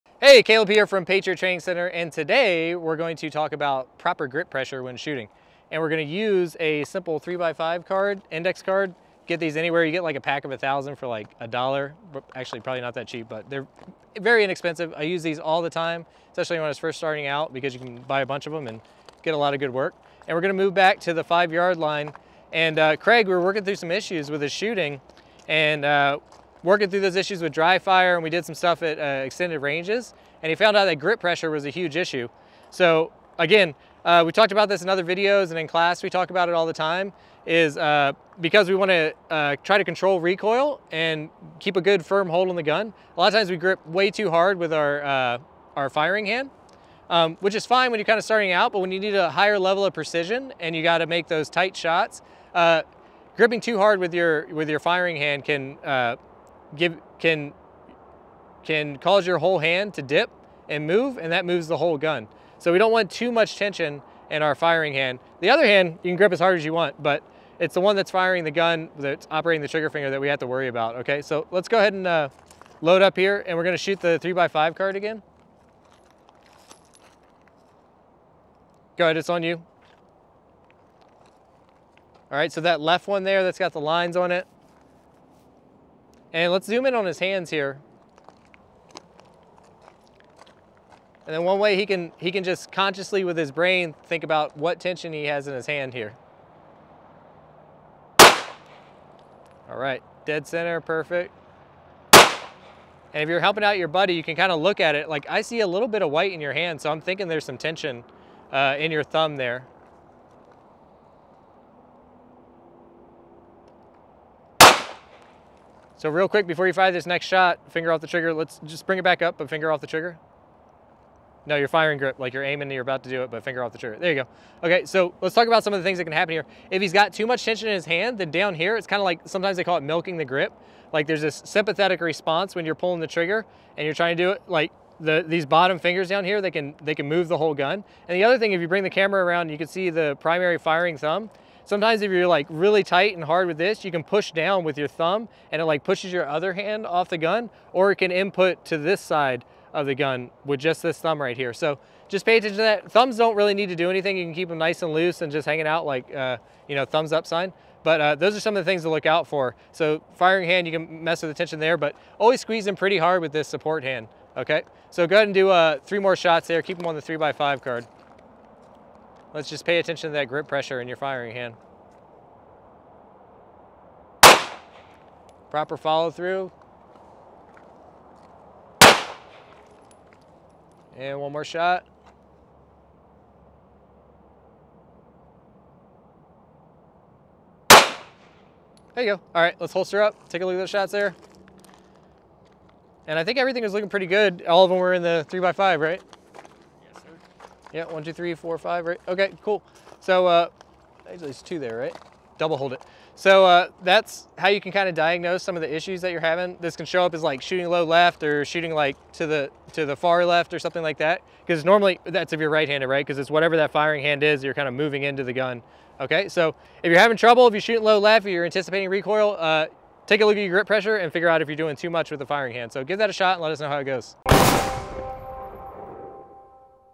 Do You Make This Common Grip Mistake?
If you're having trouble making tough shots, or you find yourself always shooting low left - you may not be anticipating recoil - it could be this simple mistake with your grip instead. Caleb, founder of Patriot Training Center walks you through how to fix it in this short video...